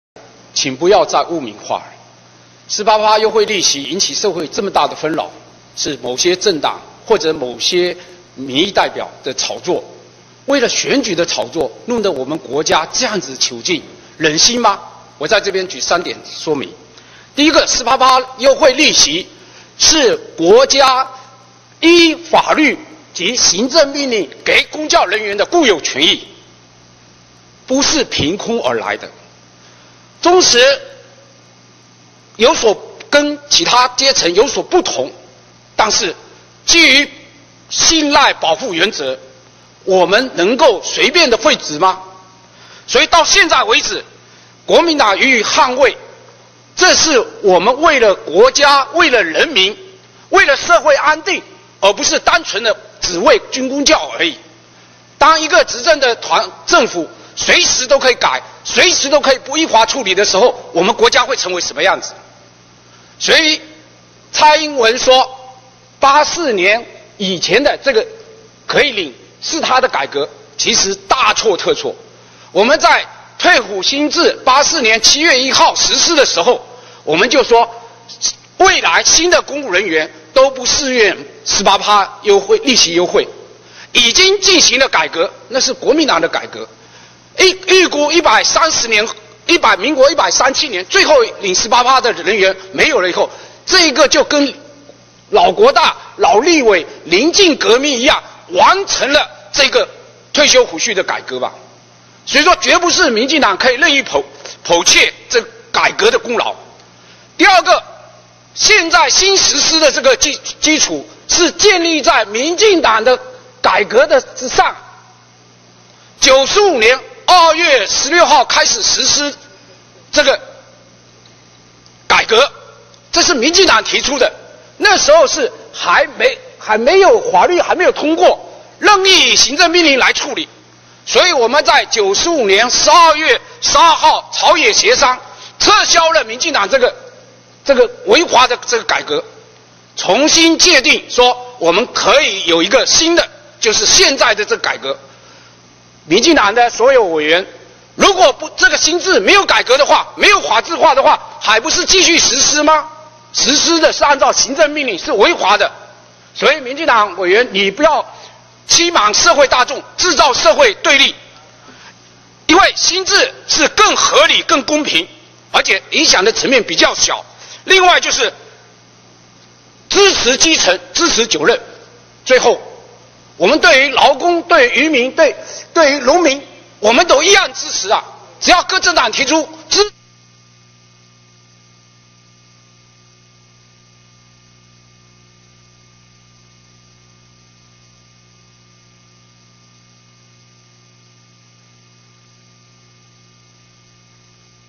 2011-01-07 曹爾忠 發言片段, 第7屆第6會期第15次會議
會議簡介: 第7屆第6會期第15次會議一、討論事項：本院財政委員會報告審查行政院函請審議「中華民國100年度中央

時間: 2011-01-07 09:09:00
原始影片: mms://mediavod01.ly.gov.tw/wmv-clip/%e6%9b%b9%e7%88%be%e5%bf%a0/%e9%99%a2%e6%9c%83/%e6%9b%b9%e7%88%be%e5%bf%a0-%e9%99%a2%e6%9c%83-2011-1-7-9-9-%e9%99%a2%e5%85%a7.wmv